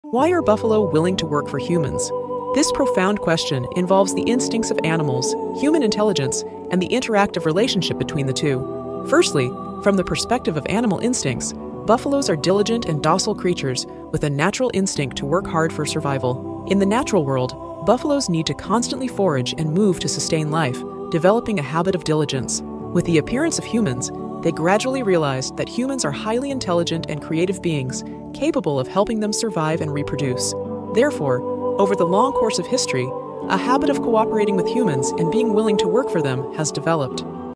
Why Buffalo Willing to Work For Hyumans
Why are buffalo willing to work for humans? This is a profound question that involves the instincts of animals, human intelligence, and the interactive relationship between the two. Firstly, from the perspective of animal instincts, buffalo are diligent and docile animals with a natural instinct to work hard for survival. In the natural world, buffalo need to constantly forage and move to sustain life, developing a habit of diligence. With the appearance of humans, they gradually realized that humans are highly intelligent and creative beings capable of helping them survive and reproduce. Therefore, over the long course of history, a habit of cooperating with humans and being willing to work for them has developed.